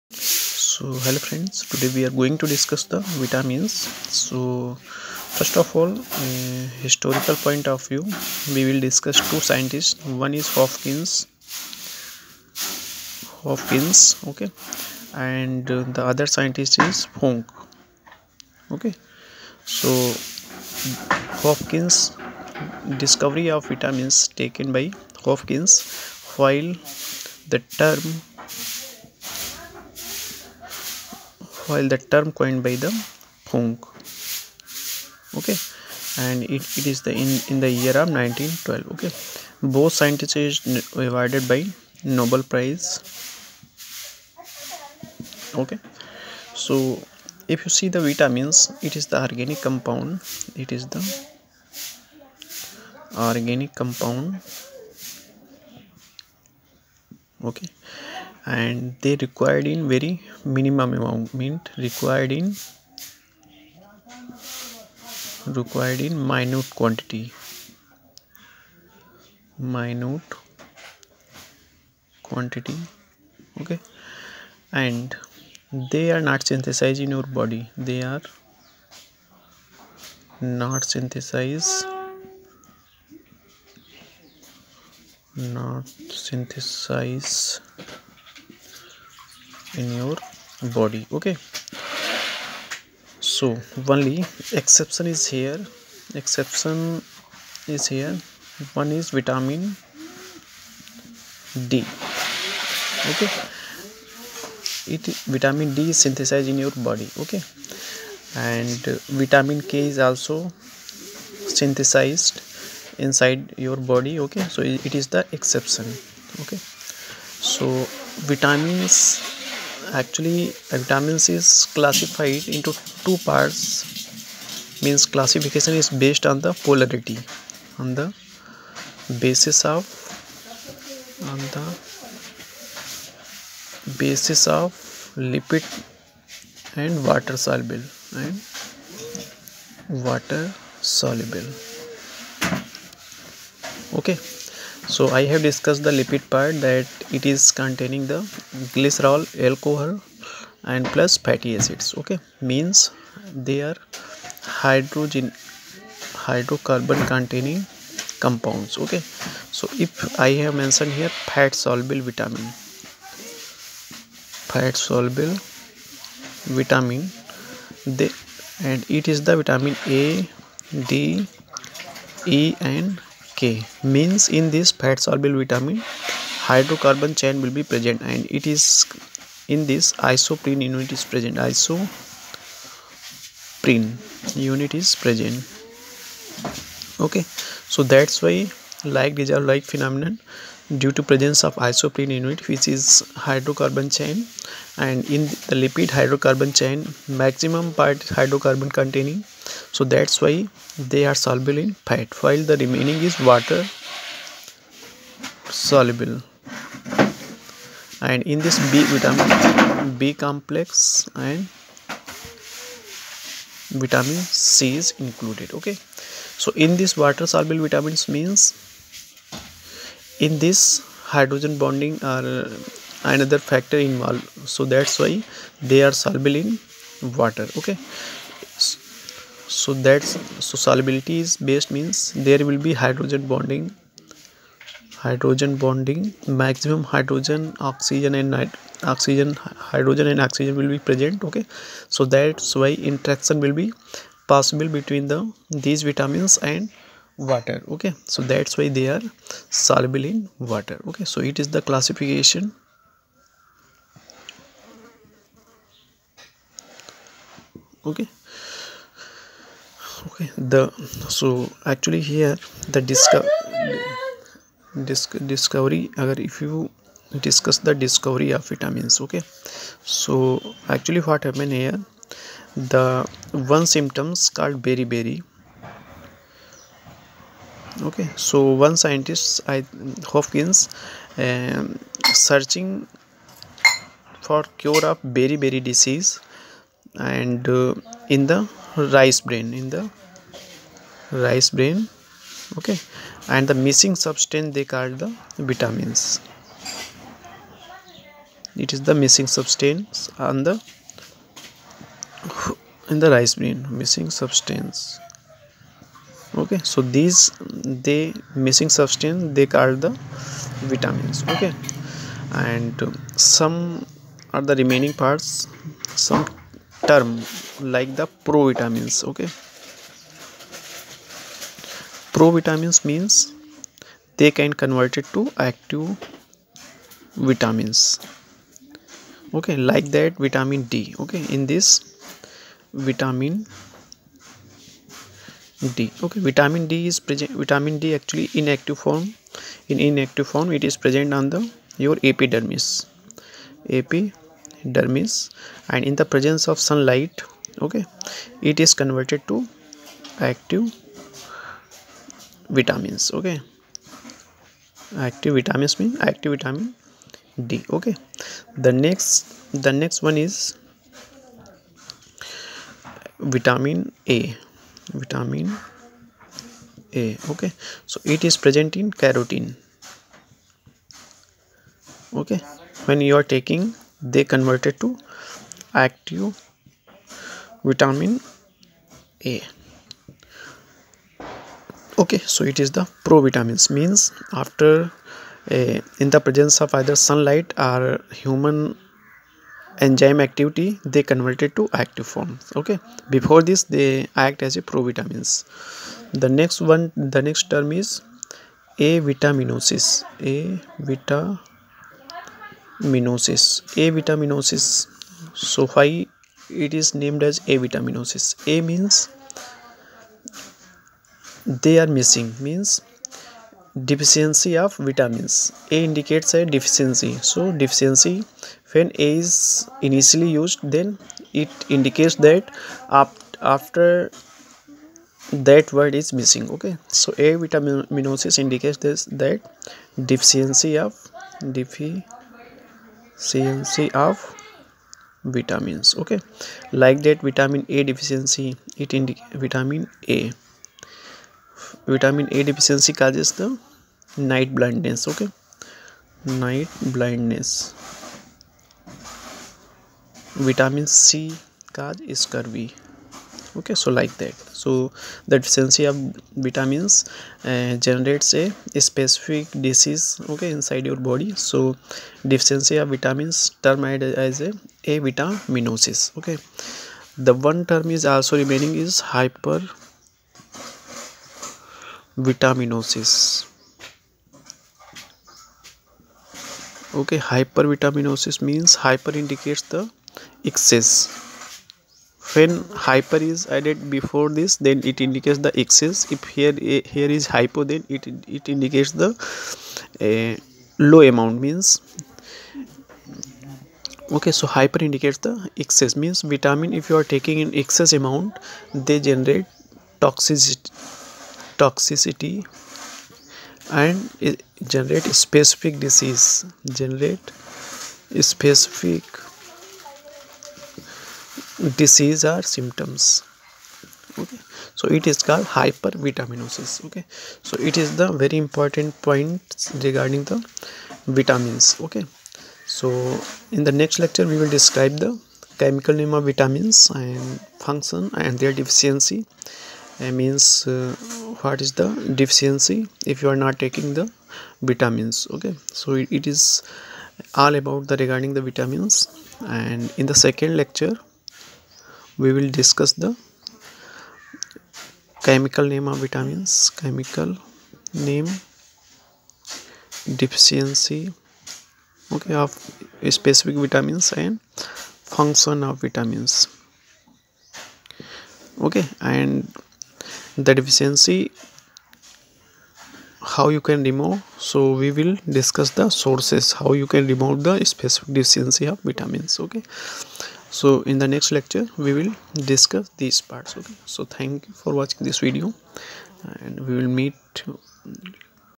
L.01 | VITAMIN A |BASIC PROPERTIES | FUNCTIONS
Vitamins are essential substances that our body needs in minute quantity, order to grow, develop normally and maintain its functions. They are not energetic compounds but needed for healthy body system.